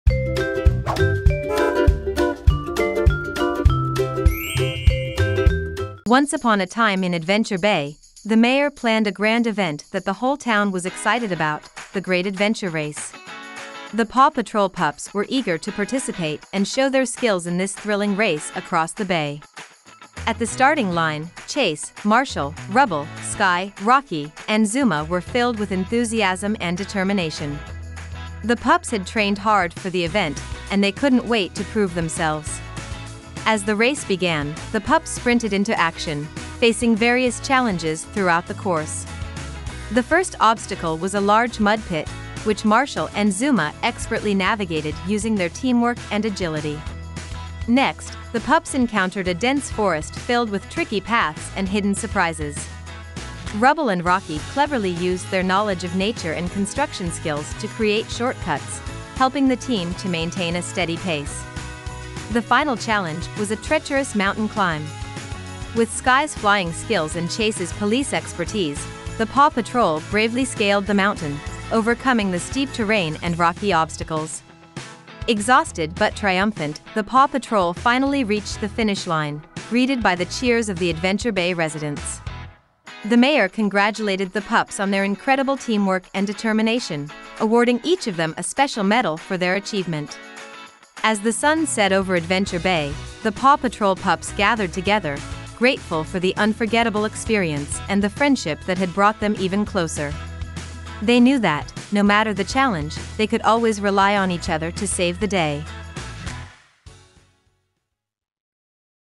🐾 Paw Patrol's Ultimate Race! 🏁 | Listen Imagine Grow 🎧
Join the Paw Patrol pups 🐶 in this exciting, action-packed race through Adventure Bay! 🏞️ Listen as Ryder leads his team of heroic pups on a thrilling journey that will keep your little ones entertained and engaged. 🚀 Tune in to Listen Imagine Grow for more amazing stories just like this one! 📚